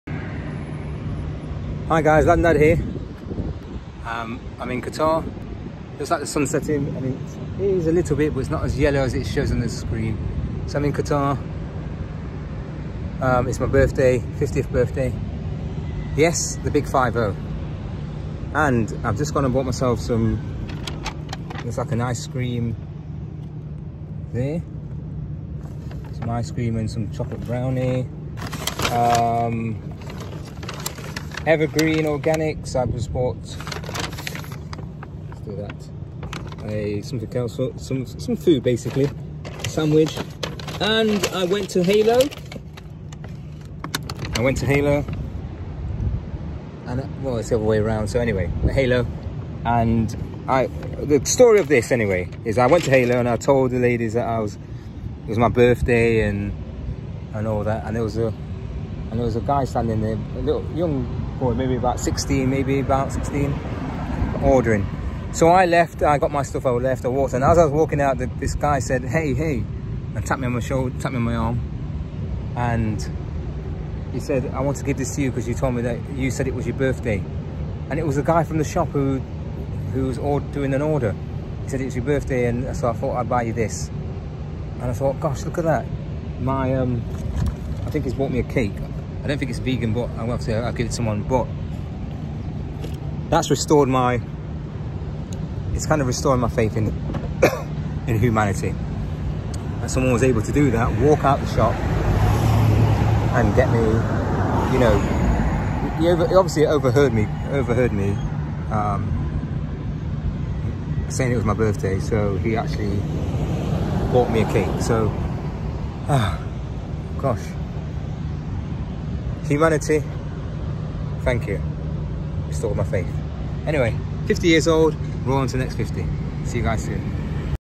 Dad is 50 years old and gets a surprise gift
Dad gets a surprise gift from a stranger